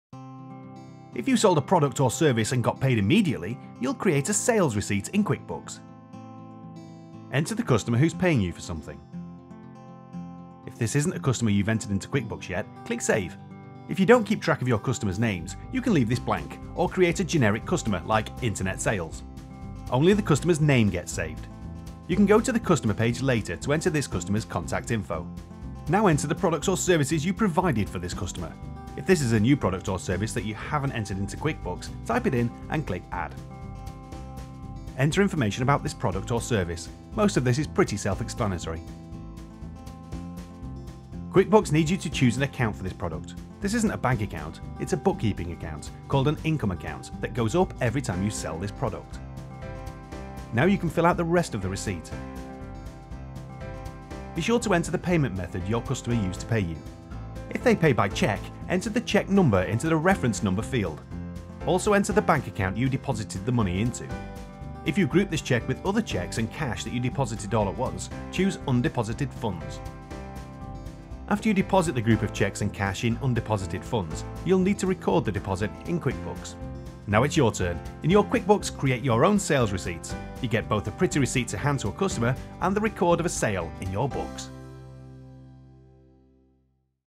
How do I create a Sales Receipt?
Find out how to create sales receipt in QuickBooks